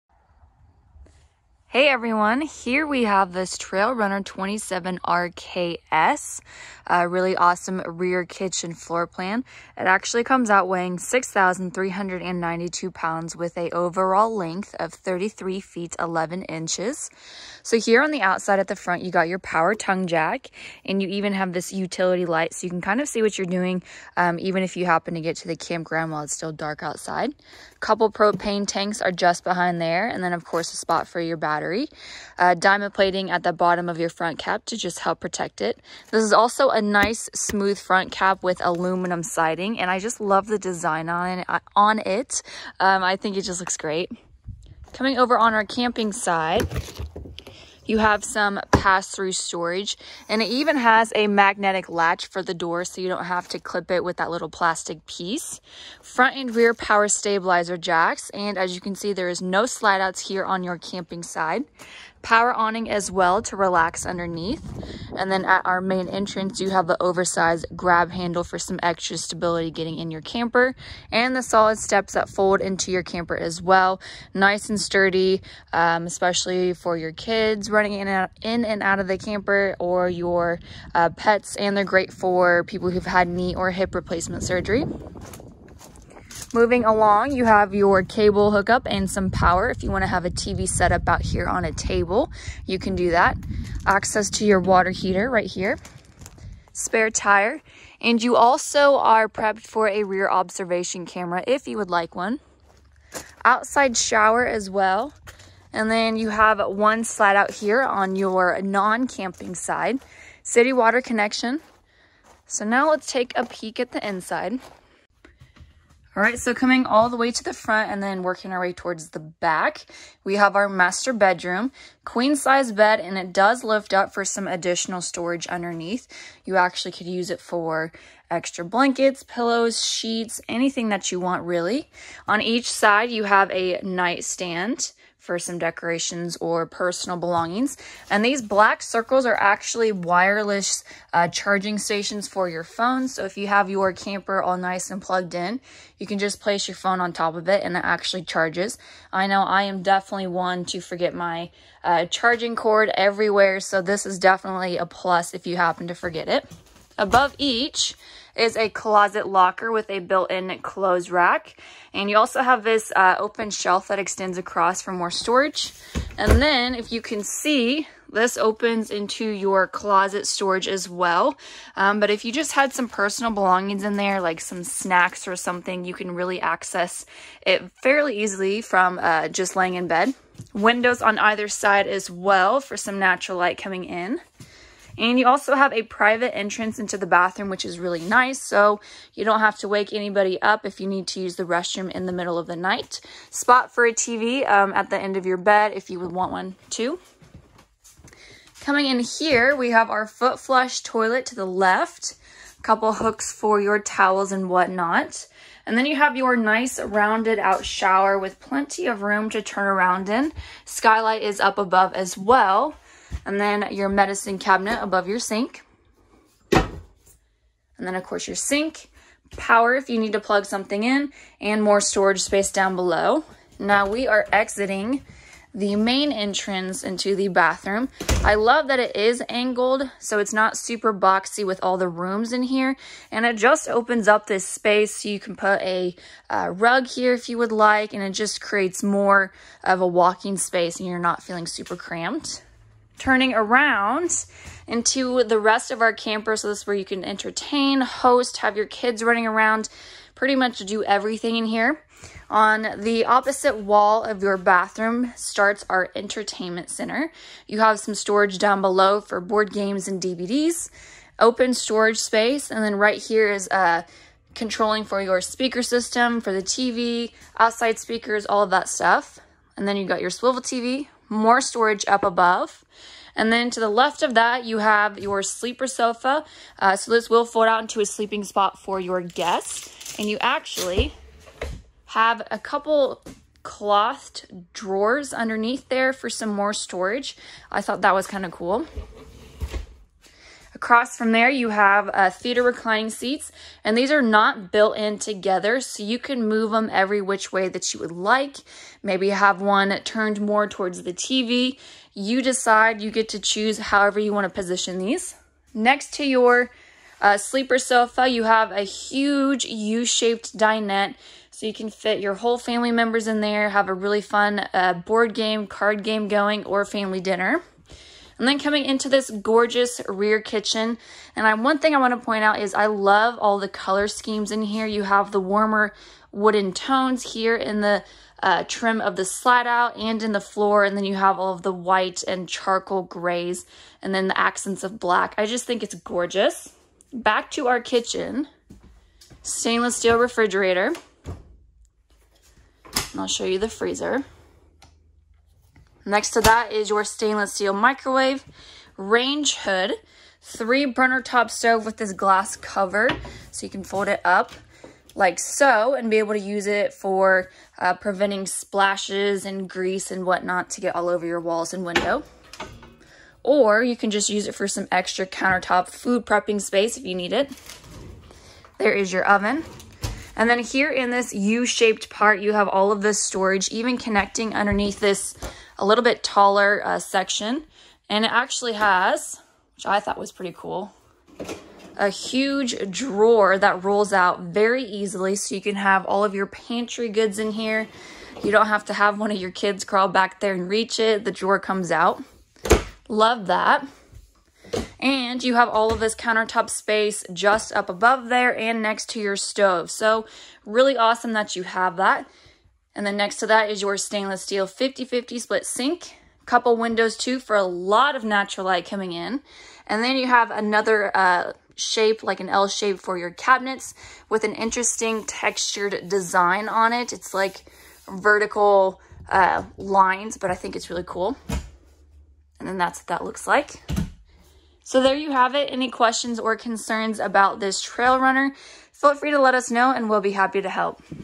Light Weight Travel Trailer | 2022 Trail Runner 27RKS
I'm obsessed with how light and bright this Trail Runner is! Makes it feel extra homey.